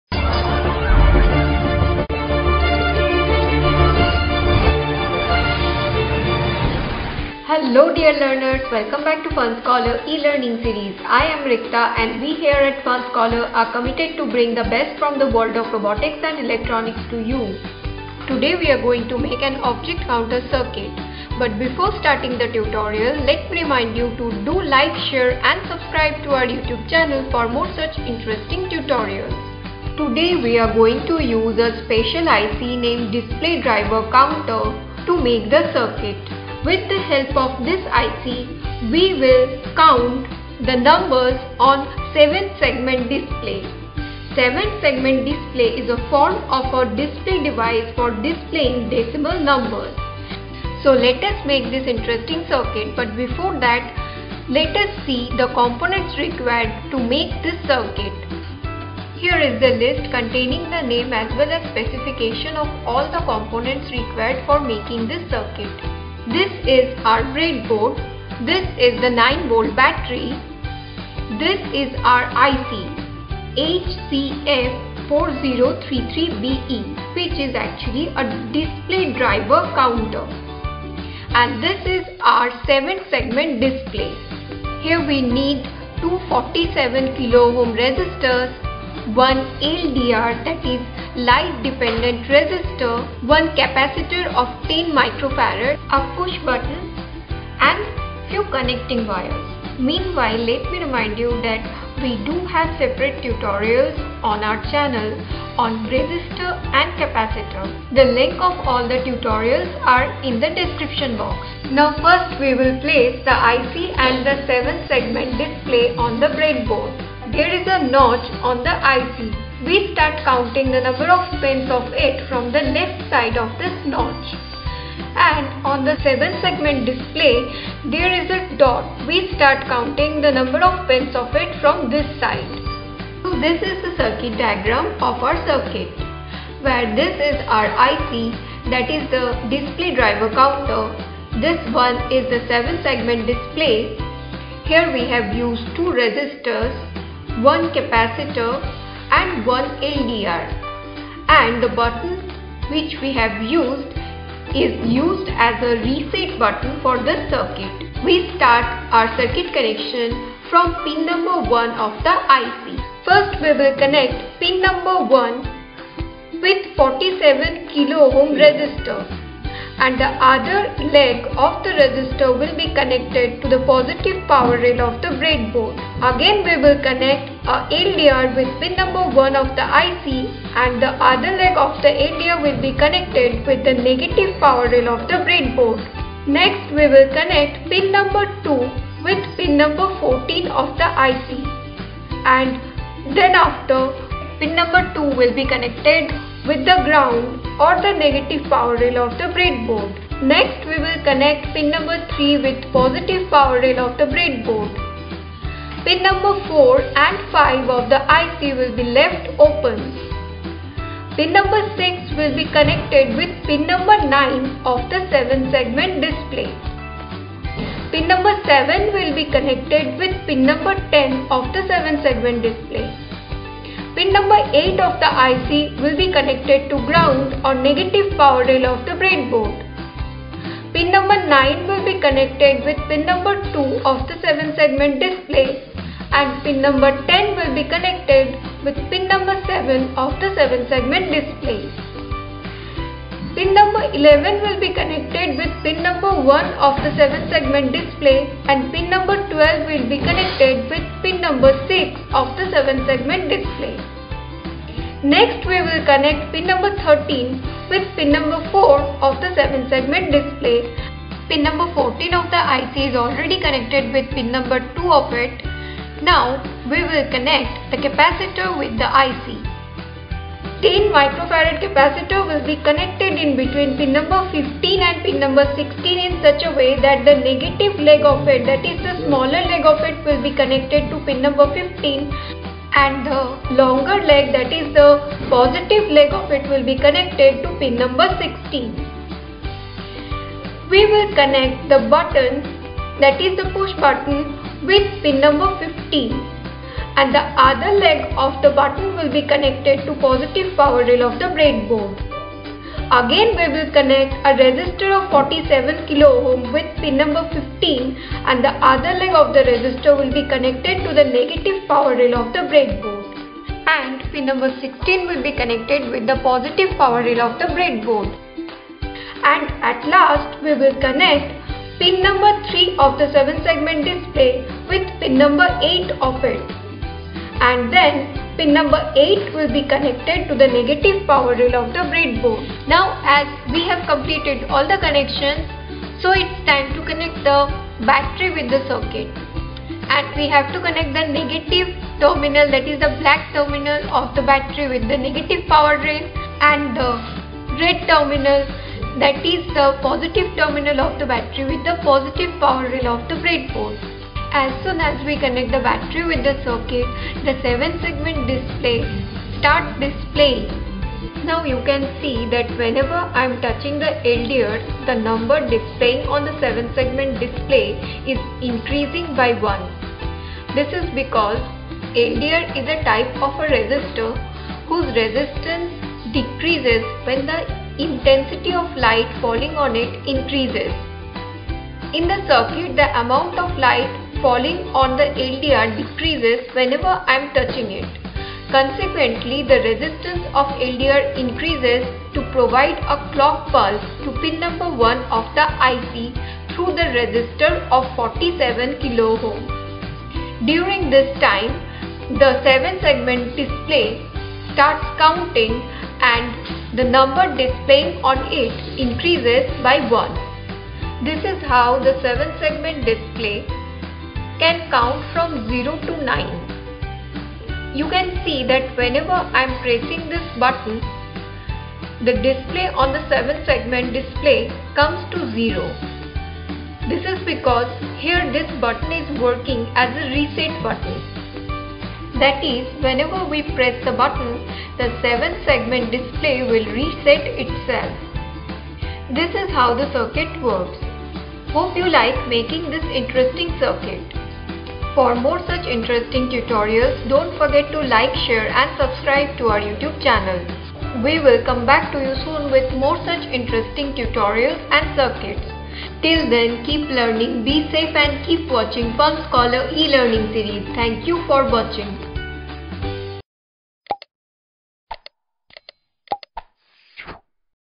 Object Counter Circuit using LDR and 4033IC
Hey There!
In this video we will learn to make an object counter circuit where LDR will help us in sending high pulse to the 4033IC which in turn controls 7 segment display increasing counter by 1.
Happy Learning...